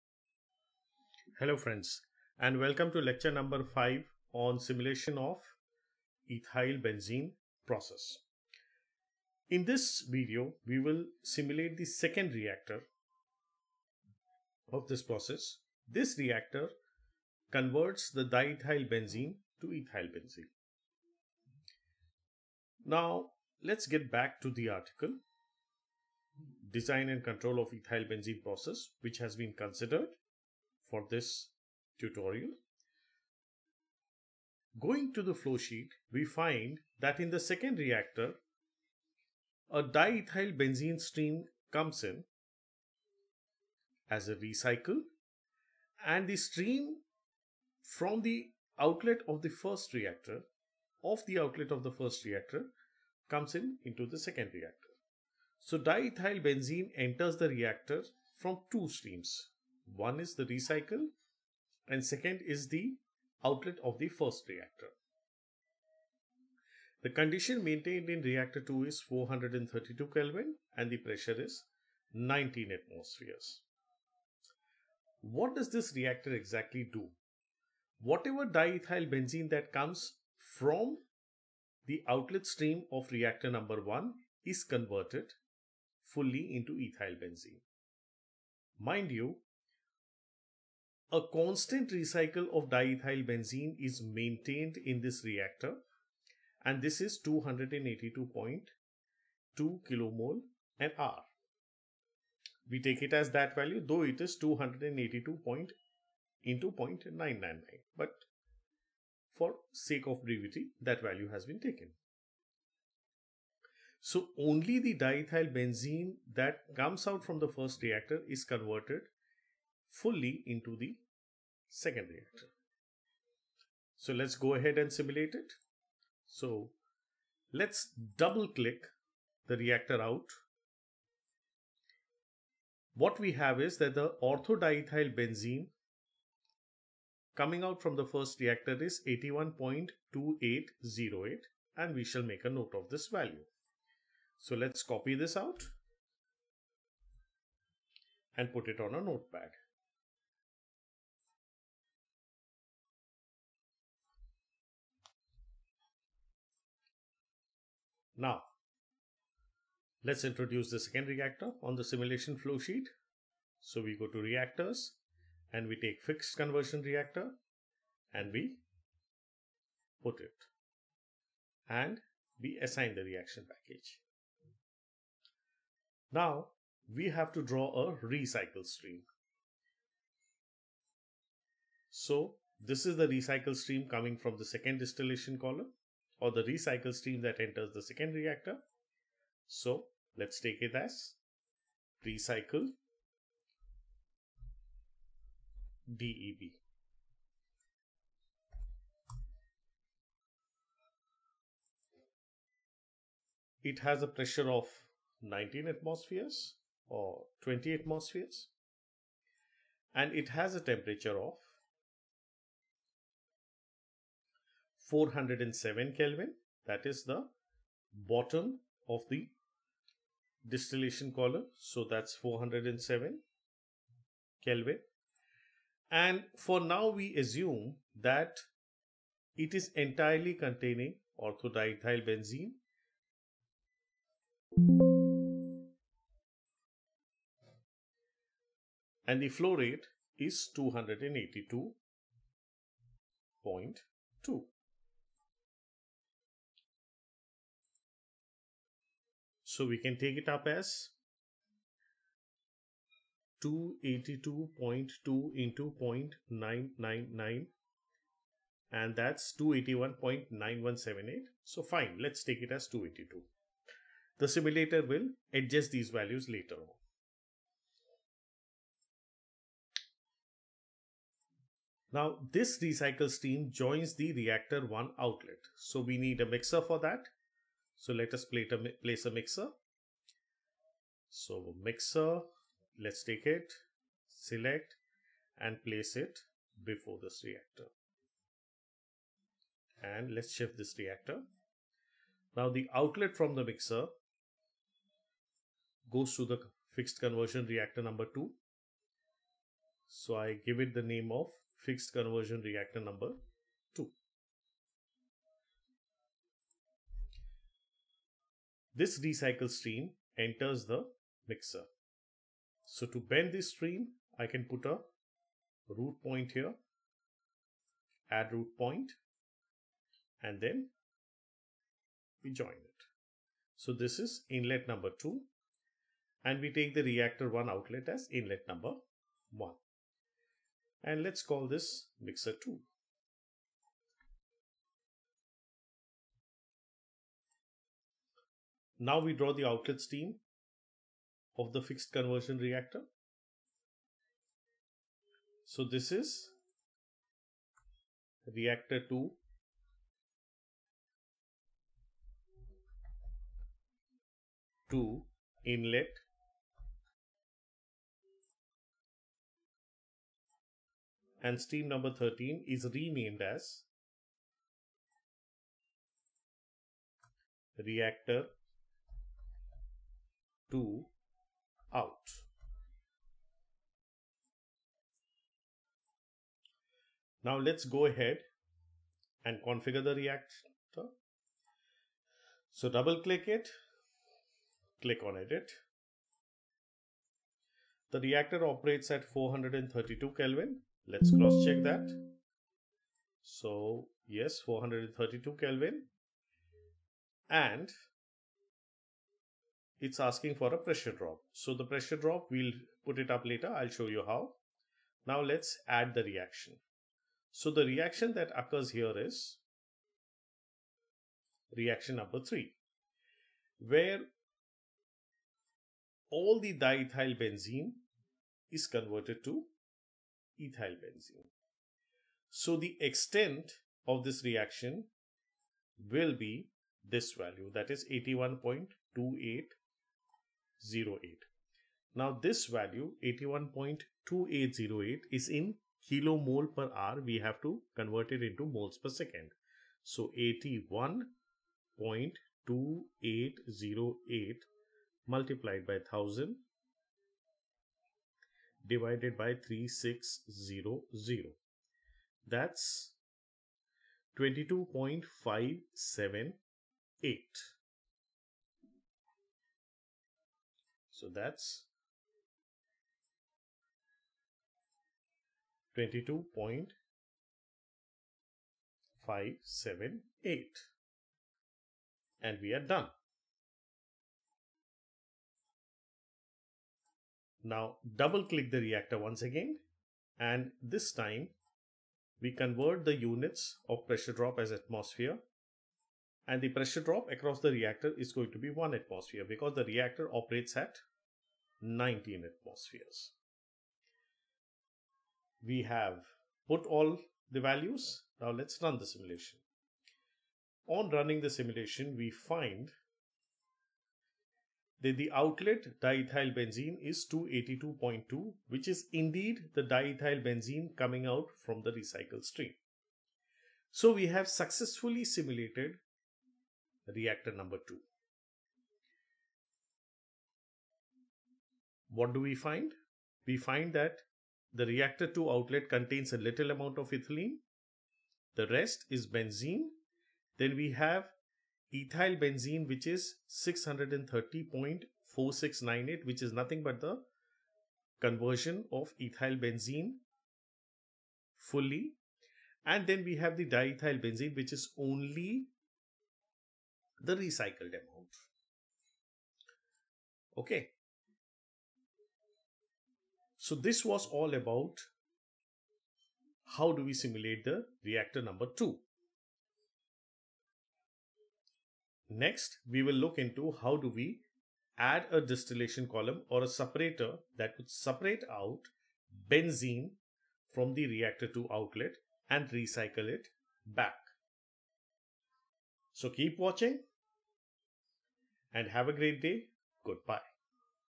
Simulation of Ethylbenzene Process_5
This tutorial shows how to simulate an reactor that converts diethyl benzene to ethylbenzene.
The flowsheet for this process is taken from the celebrated article,
This is followed by the simulation of the second reactor. This reactor converts di ethyl benzene to ethyl benzene. The simulation is shown  here